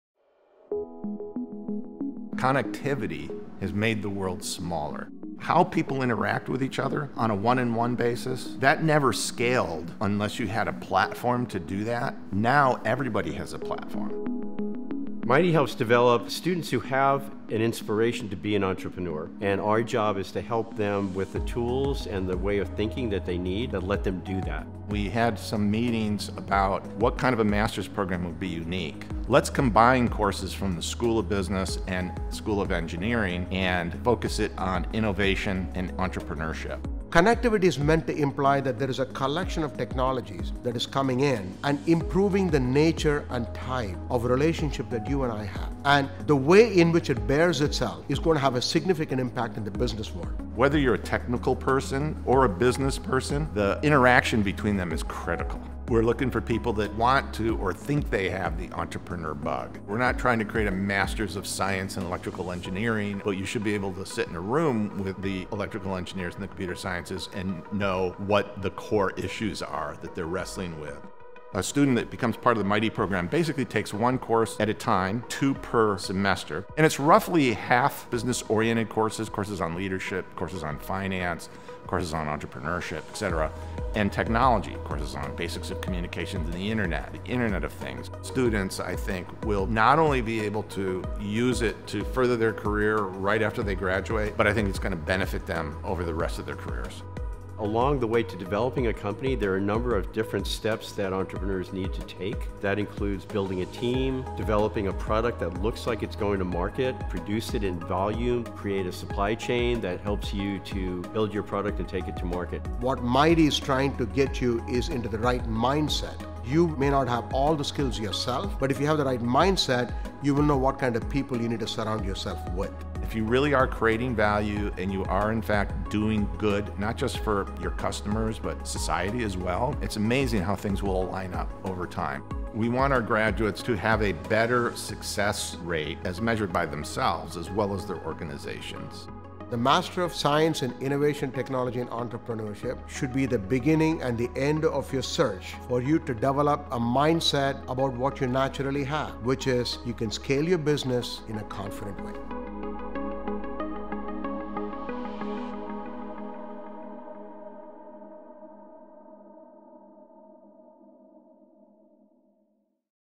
Master of Science in Innovation, Technology and Entrepreneurship | University of San Diego
The Master of Science in Innovation, Technology and Entrepreneurship program is a 33-unit project-based program with 15 units covering the technical aspects of technology innovation in the computer and communications connectivity space and 15 units with focused coaching on business, entrepreneurship and leadership. With the final three capstone units, students are placed on project teams for in-depth work on developing a specific idea or product.